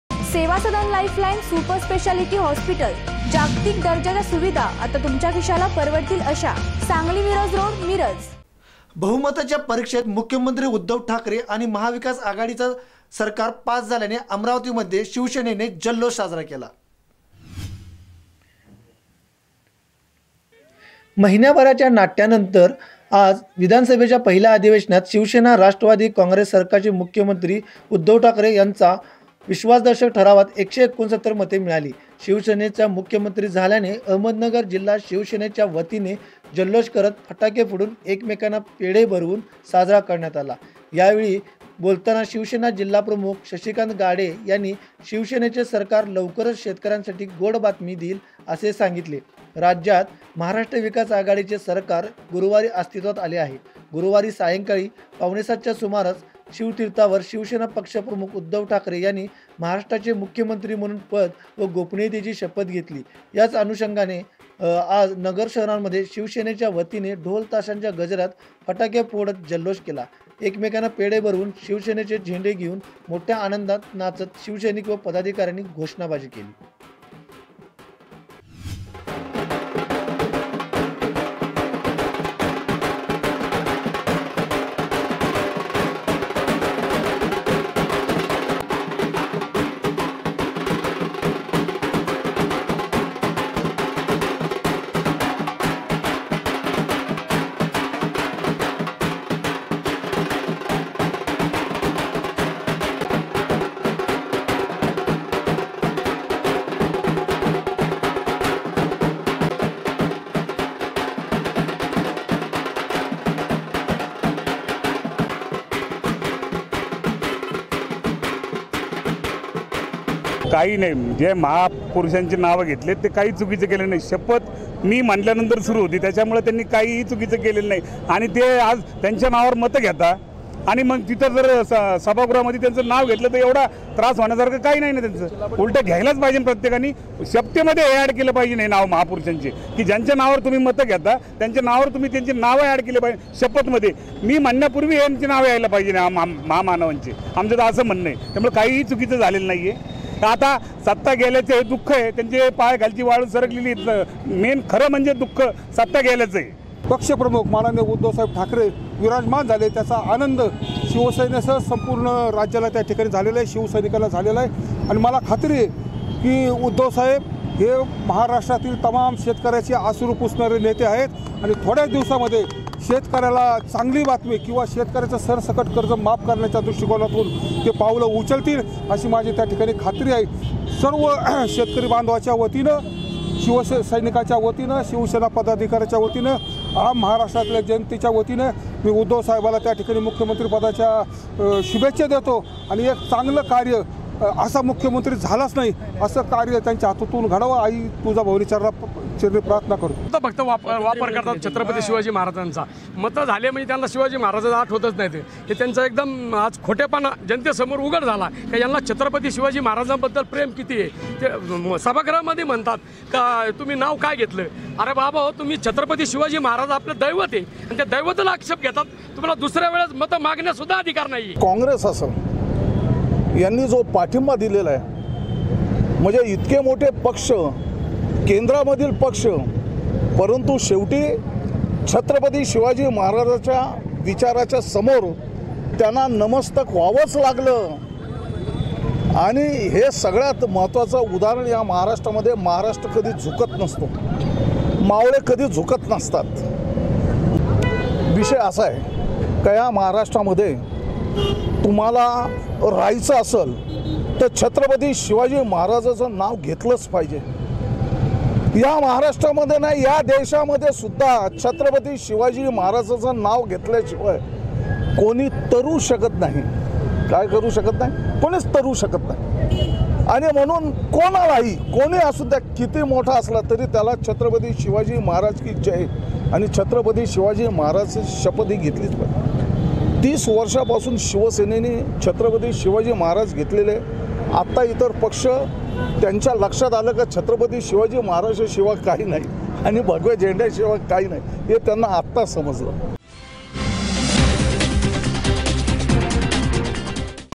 uddhav thackeray passed in floor test, celebration of shivsena in ahmadnagar
बहुमताच्या परीक्षेत मुख्यमंत्री उद्धव ठाकरे आणि महाविकास आघाडीच्या सरकार पास झाल्याने अहमदनगरमध्ये शिवसेनेचा जल्लोष.@